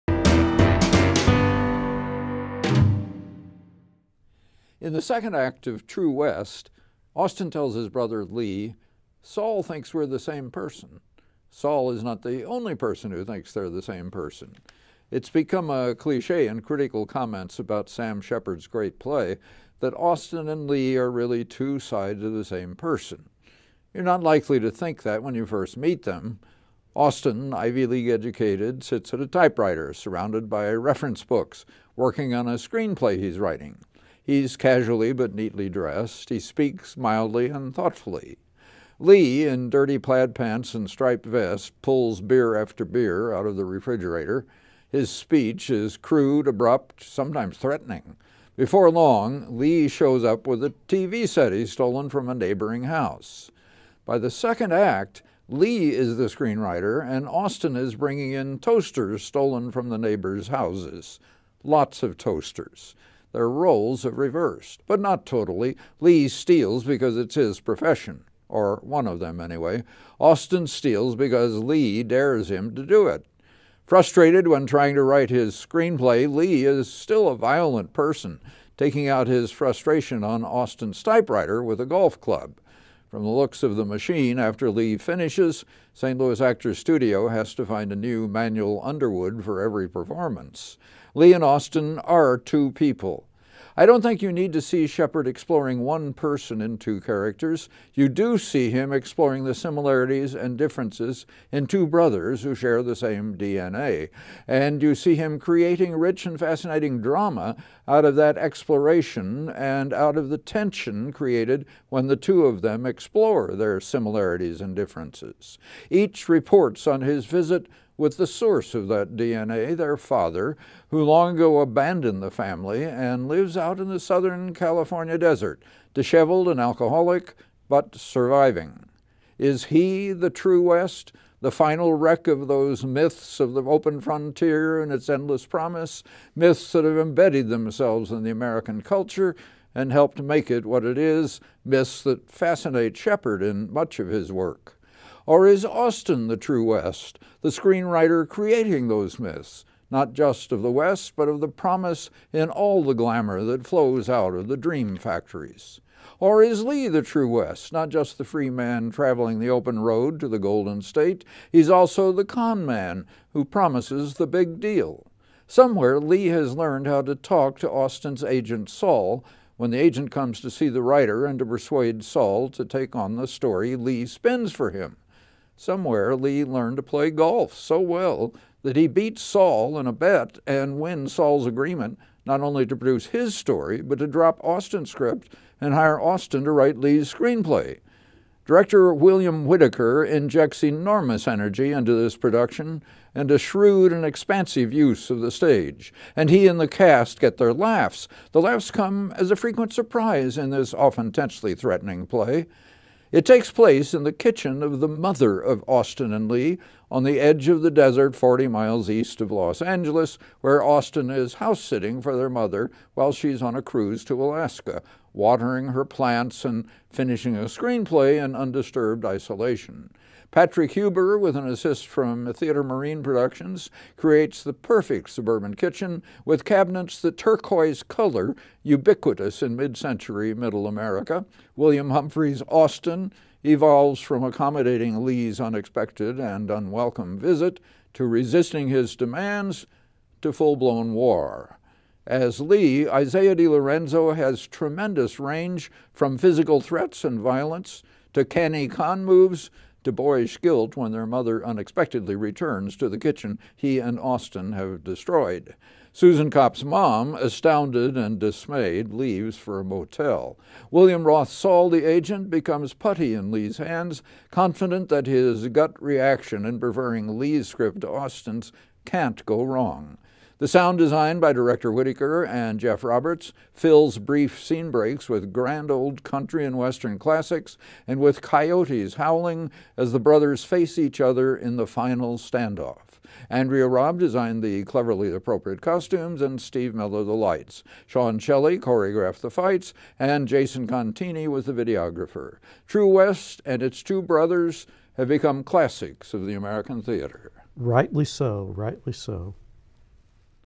True West @ St Louis Actors' Studio
A surprising number of laughs occur during the oftenly tensely threatening Sam Shepard play “True West”. The players at St. Louis Actors’ Studio took on this American theatre classic in the Central West End and Bob Wilcox has the review. Just click here to check it out.